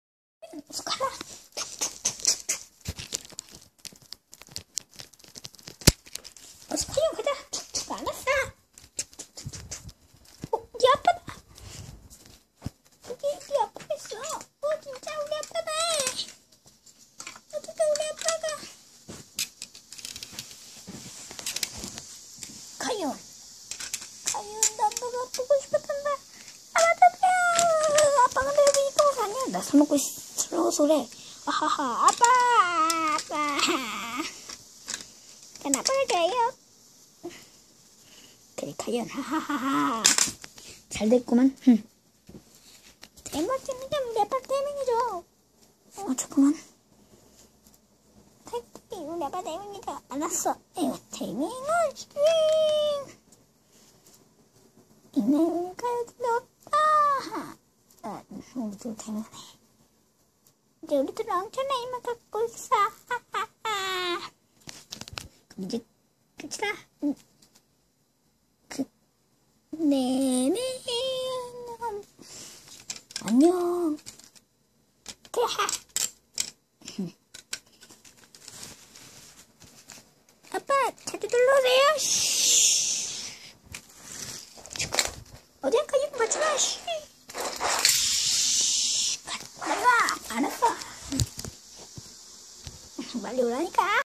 카이온
카이온과내가윙라이온을찾으러간다.